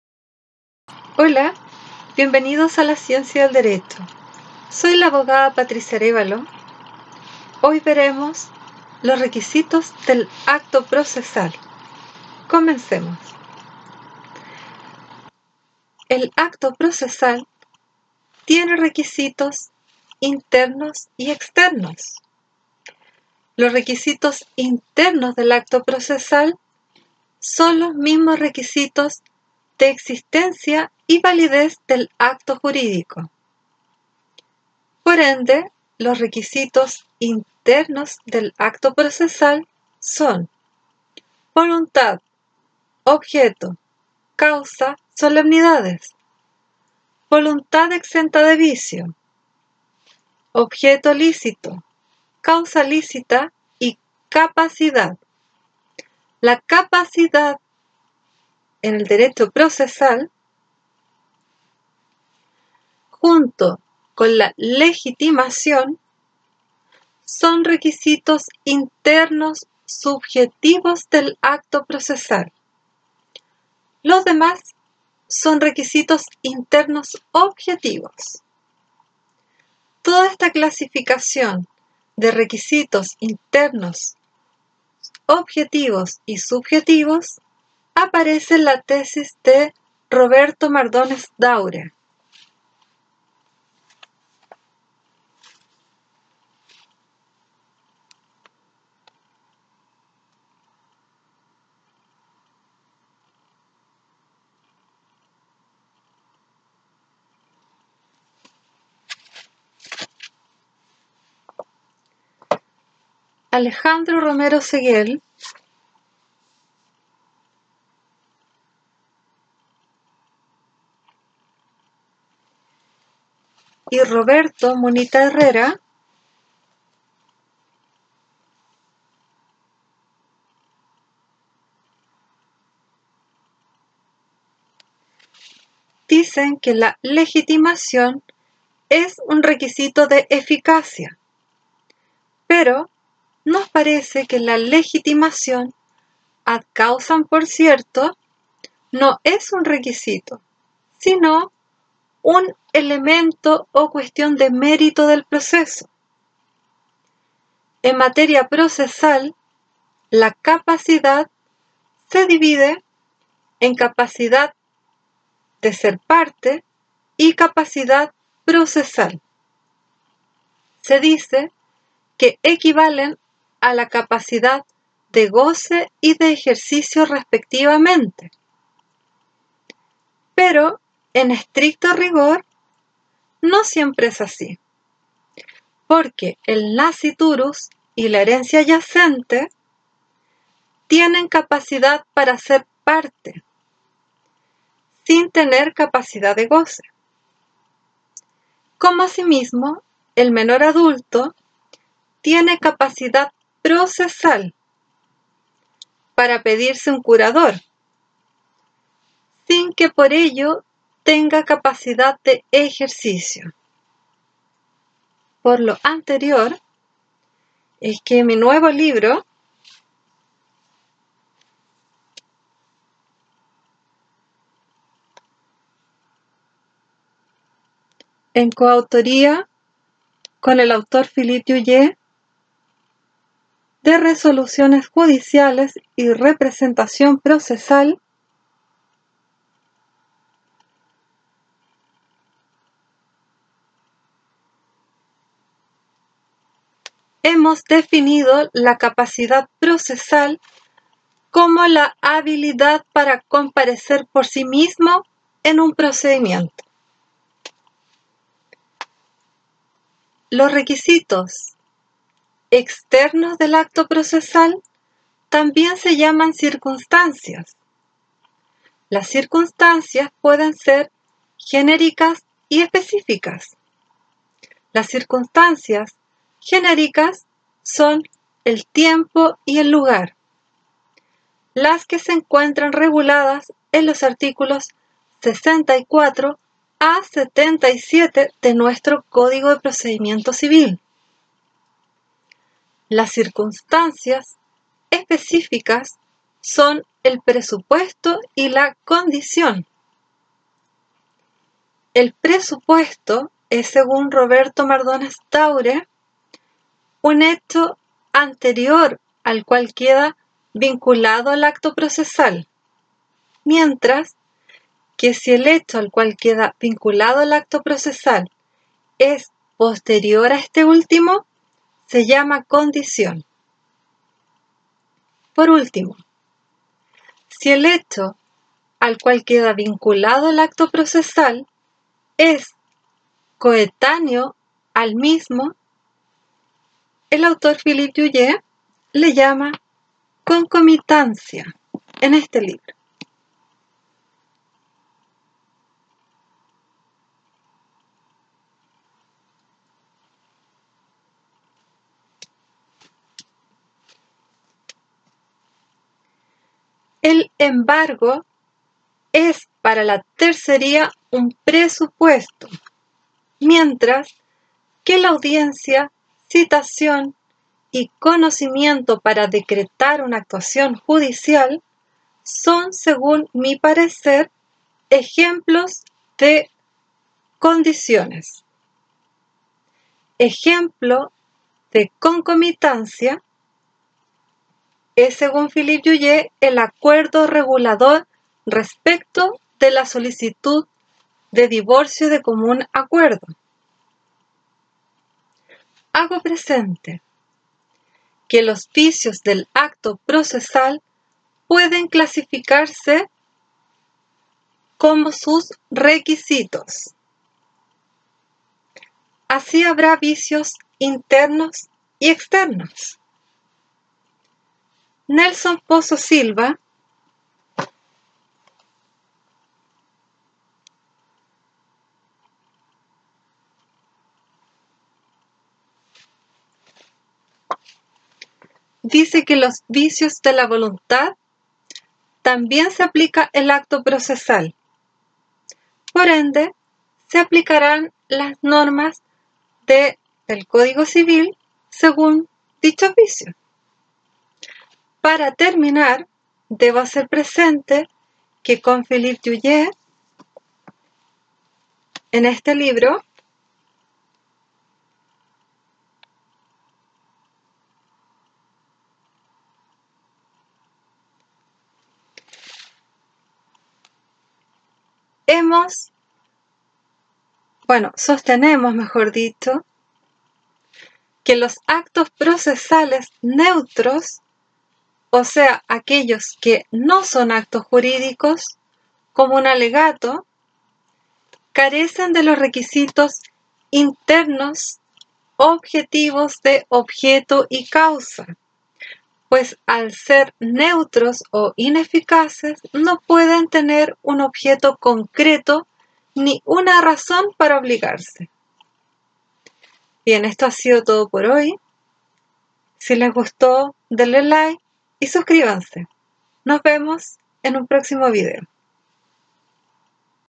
Requisitos del acto procesal - Clase 16 de Derecho Procesal Chile
BIBLIOGRAFÍA: 1.- MARDONES DAURE, Roberto, La Nulidad Procesal por la vía Incidental, Memoria  de  Prueba, Editorial Universitaria S.A., Santiago, Chile, año 1962, N° 15, pág. 27.
2.- ROMERO SEGUEL, Alejandro, Curso de Derecho Procesal, Santiago, Chile, 2ª Edición, Editorial  LegalPublishing, año 2014, Tomo I, pág. 89.
3.-MUNITA  HERRERA,  Roberto, Estudio Crítico sobre el Recurso de Queja, Santiago, Chile, Editorial Jurídica de Chile, año 1968, pág. 20.
4.- LLULLE NAVARRETE, Philipe, Tratado del divorcio, de la compensación económica y de las indemnizaciones entre cónyuges y convivientes civiles, Santiago, Chile, Editorial El Jurista, 2ª edición, año 2018, N° 65, pág. 138.
5.- POZO SILVA, Nelson, De las nulidades procesales, Santiago, Chile, 5ª Edición, Editorial ConoSur Ltda., año 2001, pág. 45.
6.- ARÉVALO AYALA, Patricia y LLULLE NAVARRETE, Philipe, Resoluciones Judiciales y Representación Procesal, Santiago, Chile, Editorial El Jurista, año 2020, N° 11, págs. 56 a 61. La definición de capacidad procesal está en la pág. 59 y la opinión sobre falta de objeto y causa en los actos procesales neutros está en la pág. 61.